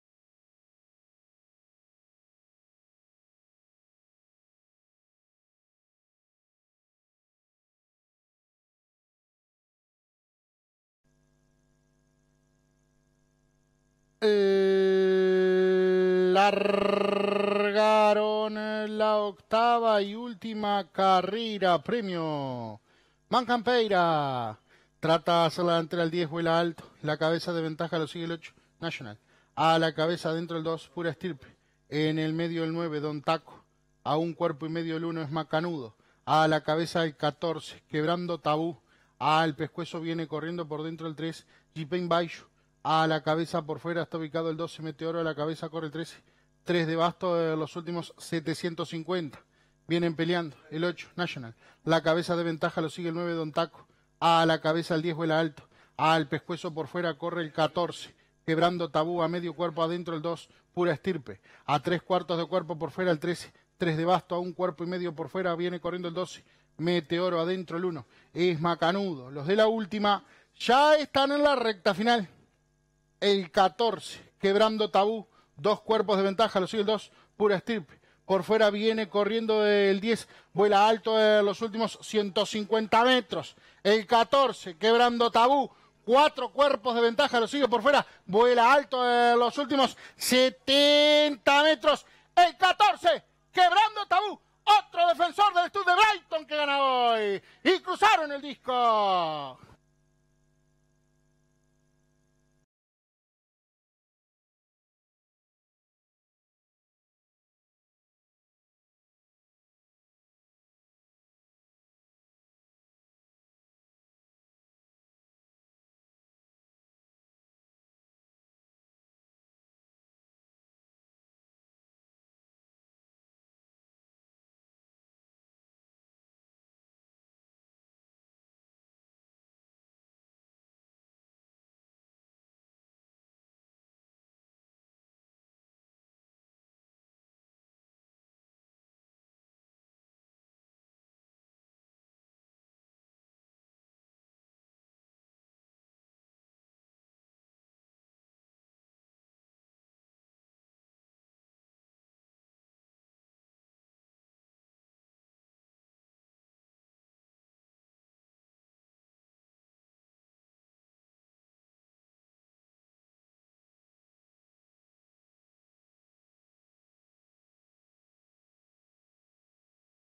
11/11/2022 – Hipodromo Las Piedras – Carr 8 – MAN CAMPEIRA (2021)
Carrera 8 realizada el dia 11/11/2022 por el premio MAN CAMPEIRA (2021) en el Hipodromo Las Piedras